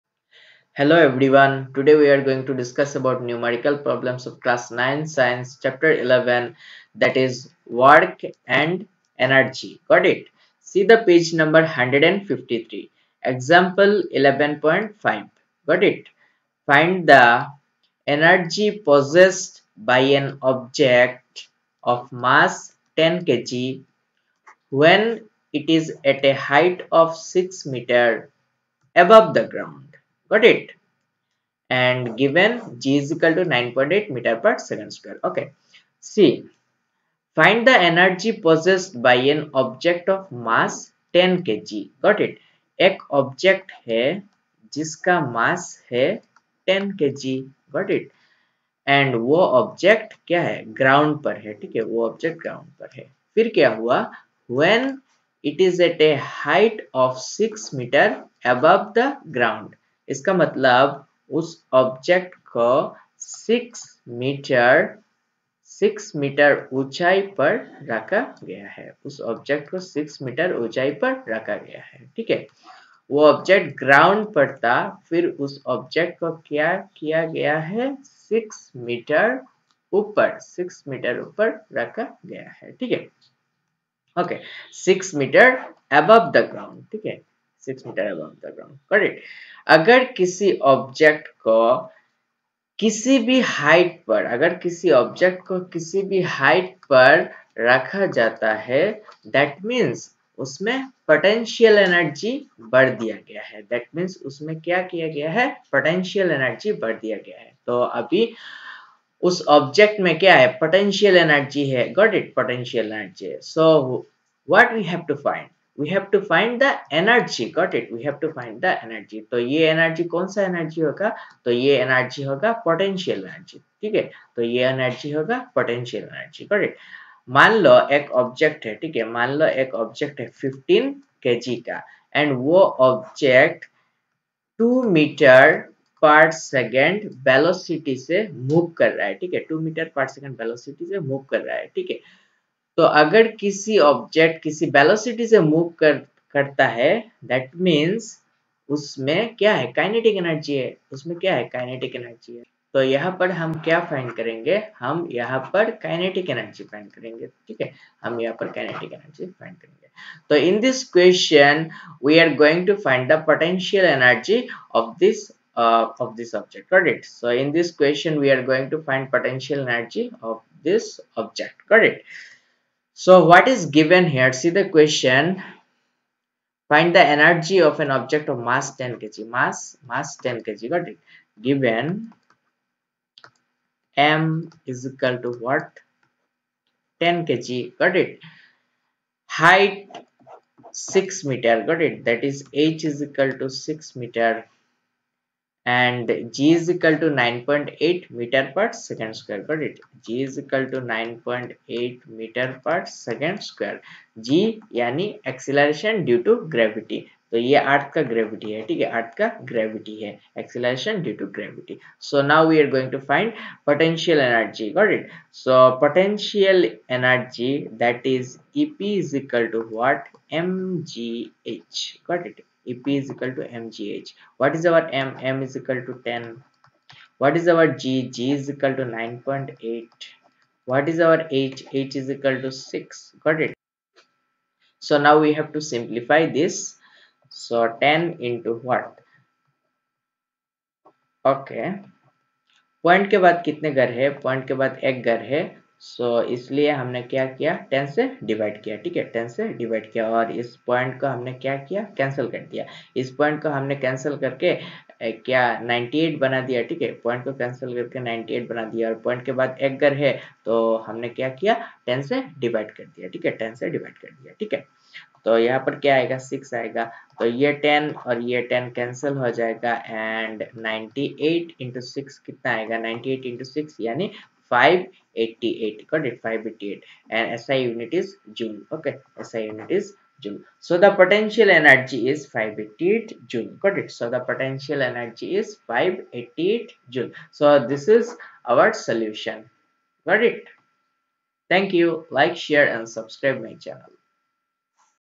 Class 9th Science Chapter 11 | Example 11.5 | Work and Energy | NCERT SEBA CBSE
This video includes step by step explanation of example 11.5 of chapter 11 (work and energy).